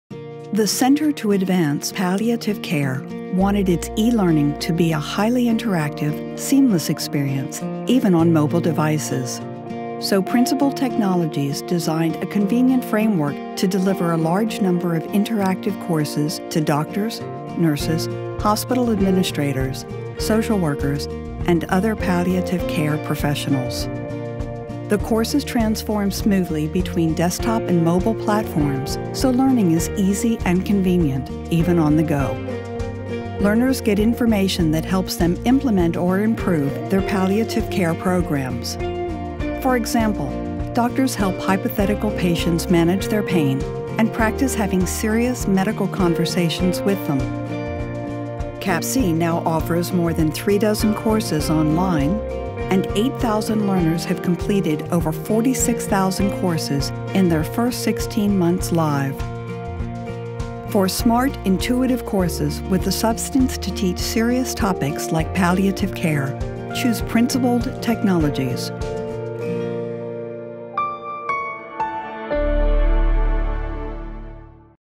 E-learning: Healthcare Online Curriculum
This series of interactive courses trains doctors, nurses, and other healthcare professionals on various palliative care topics. PT designed the courses to be responsive and function well on both desktops and mobile devices, so learners can train on the go.